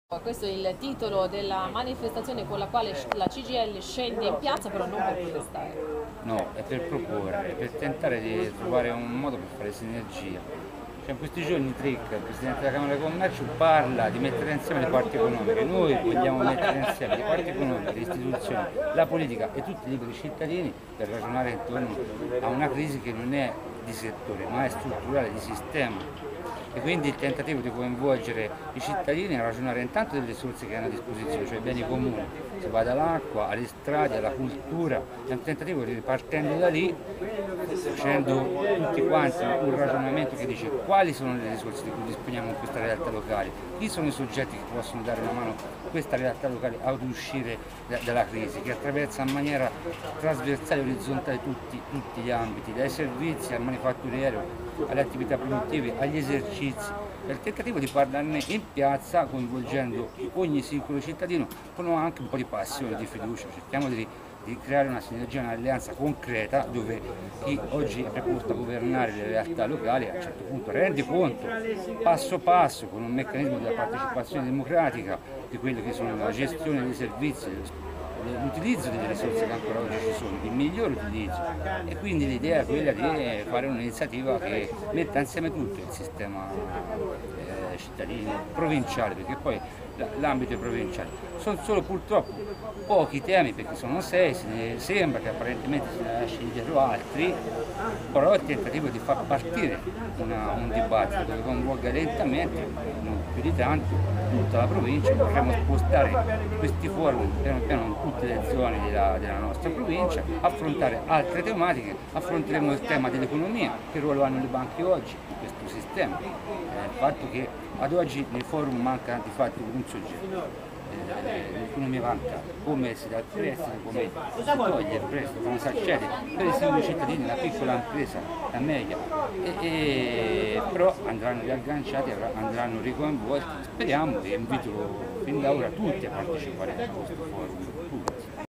PRESENTAZIONE FORUM SOCIALE
Il 25 ed il 26 maggio appuntamento in piazza Risorgimento e alla borsa merci di Arezzo con il Forum Sociale "Io ti pubblico"